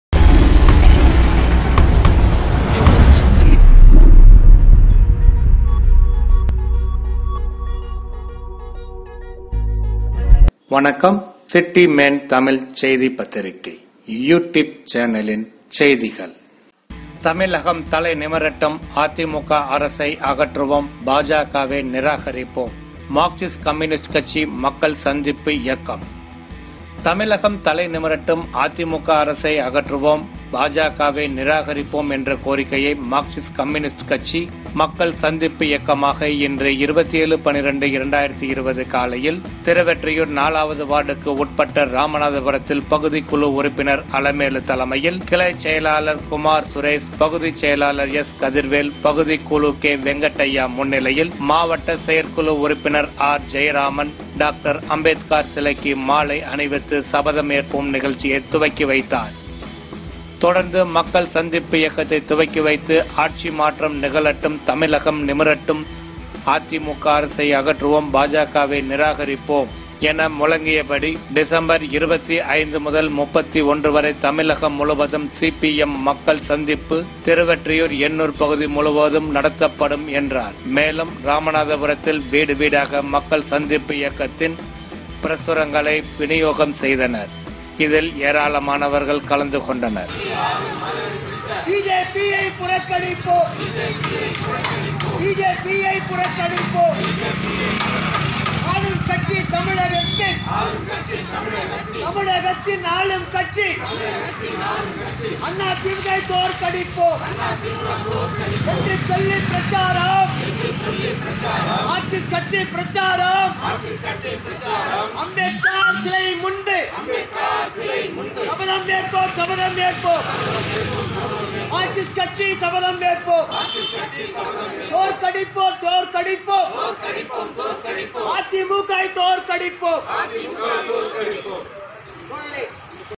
தமிழகம் தலை நிமிரட்டும் அதிமுக பாஜகவை நிராகரிப்போம் CPI M மக்கள் சந்திப்பு இயக்கம் 27.12.2020
தமிழகம் தலை நிமிரட்டும் அதிமுக அரசை அகற்றுவோம் பாஜகவை  நிராகரிப்போம்
மார்க்சிஸ்ட் கம்யூனிஸ்ட் கட்சி மக்கள் சந்திப்பு இயக்கம் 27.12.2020
தமிழகம் தலை நிமிரட்டும் அதிமுக அரசை அகற்றுவோம்   பாஜகவை
நிராகரிப்போம் என்ற கோரிக்கையை மார்க்சிஸ்ட் கம்யூனிஸ்ட் கட்சி மக்கள் சந்திப்பு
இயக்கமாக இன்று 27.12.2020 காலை
திருவொற்றியூர் 4வது வார்டுக்கு உட்பட்ட ராமநாதபுரத்தில் பகுதி குழு உறுப்பினர் அலமேல்
தலைமையில்   கிளைச் செயலாளர்  குமார் சுரேஷ் பகுதி செயலாளர்  எஸ். .கதிர்வேல்
பகுதி குழு கே .வெங்கட் ஐயா  முன்னிலையில் மாவட்ட செயற்குழு உறுப்பினர் ஆர் .ஜெயராமன்
டாக்டர் அம்பேத்கர் சிலைக்கு மாலை அணிவித்து சபதம் ஏற்போம் நிகழ்ச்சி  துவக்கி வைத்த ர்

தொடர்ந்து மக்கள் சந்திப்பு இயக்கத்தை துவக்கி வைத்து  ஆட்சி மாற்றம் நிகழட்டும் -
தமிழகம் நிமிரட்டும்... அதிமுக அரசை அகற்றுவோம். பாஜகவை நிராகரிப்போம்...
டிசம்பர் 25 முதல் 31 வரை தமிழகம் முழுவதும் சிபிஎம் மக்கள் சந்திப்பு திருவெற்றியூர்
எண்ணூர் பகுதி முழுவதும் நடத்தப்படும். என்றார். மேலும்
ராமநாதபுரத்தில் வீடு வீடாக மக்கள் சந்திப்பு இயக்கத்தில் பிரசுரங்களை விநியோகம்
செய்தனர்.இதில் ஏராளமானவர்கள் கலந்து கொண்டனர்